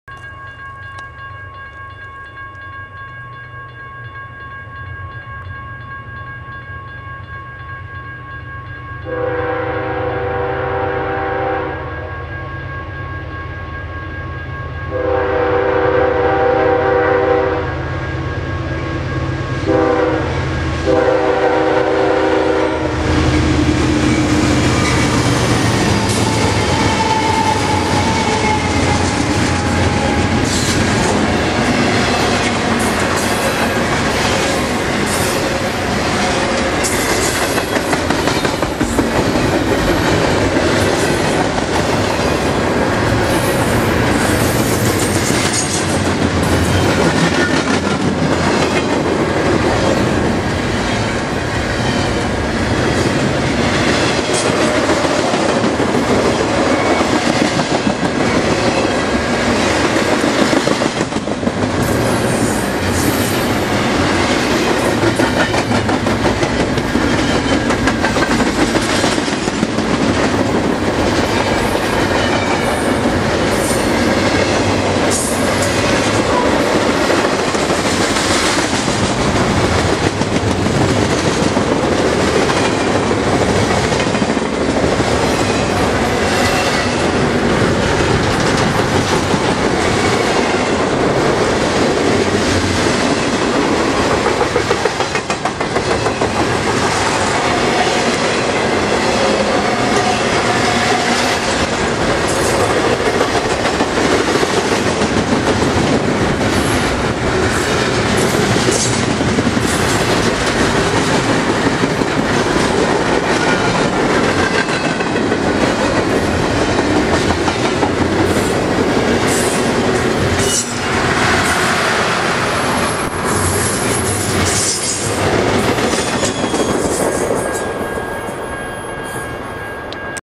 CSX 5450 Leads CSX Q127-19 at South Tunnel Hill
12/20/2009
3:02 P.M. ET
CSX W&A Sub
South Tunnel Hill

Tunnel Hill, GA

CSX Q127 rolls south through Tunnel Hill with 5500 ft of train.

CSX 7627 C40-8
CSX 7577 C40-8